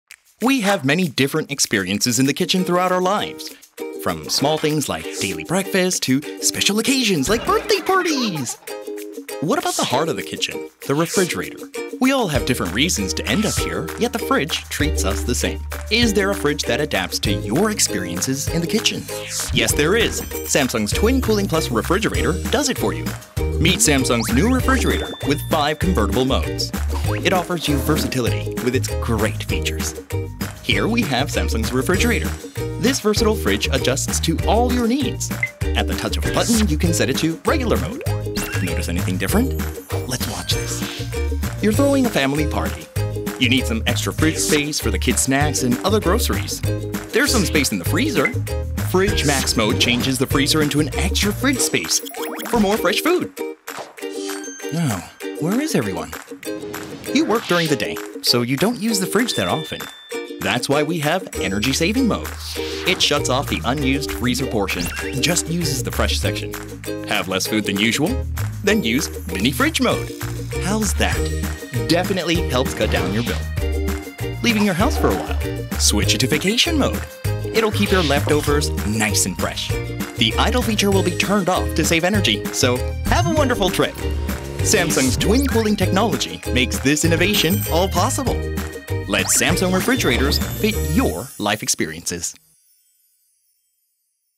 Twin Cooling 5 Conversion Modes
Life is easier with Samsung TwinCooling Refrigerator technology!

 Samsung's latest twin cooling technology offers you the best in life as it adapts to your daily needs. Saves energy, saves your electricity bills, and get more space whenever you need with its 5 conversion modes.